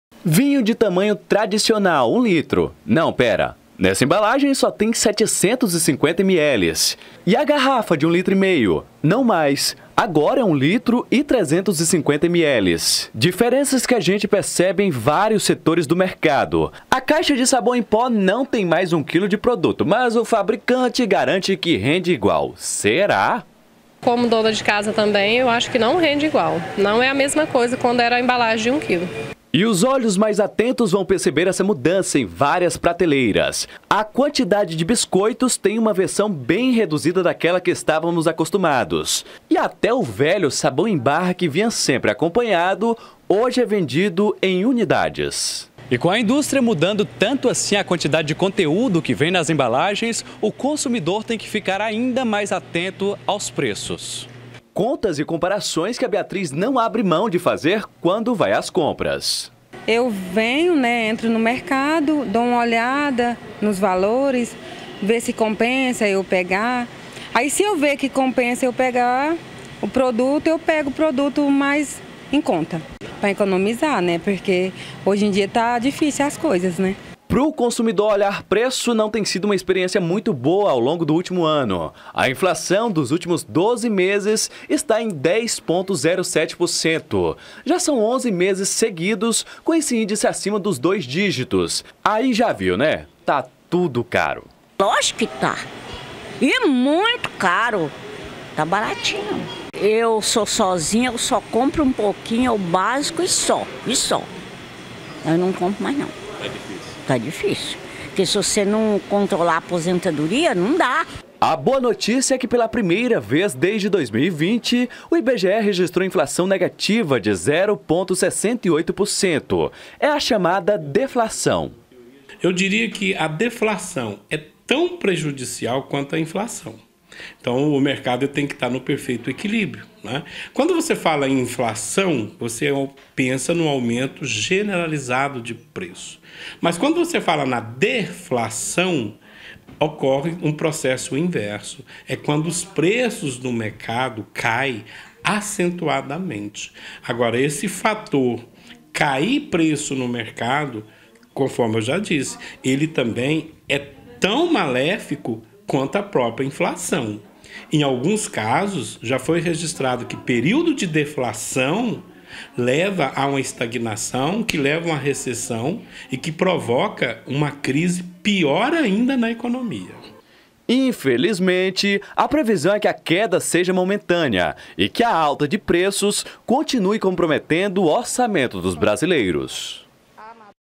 VR REDUFLAÇAO JATAI
REDUFLAÇÃO - EMBALAGENS MENORES NÃO FREIAM ALTA NOS PREÇOS
A GENTE VAI FALAR AGORA DE REDUFLAÇÃO.//VOCÊ SABE O QUE É ISSO? MESMO SEM CONHECER O NOME VOCÊ JÁ DEVE TER SIDO VÍTIMA DESTE PROCESSO.// É QUANDO O PRODUTO QUE ESTÁ CARO REDUZ O TAMANHO E OS CONSUMIDORES SEGUEM COMPRANDO, MAS LEVANDO CADA VEZ MENOS.// MARCOS GASPAR.//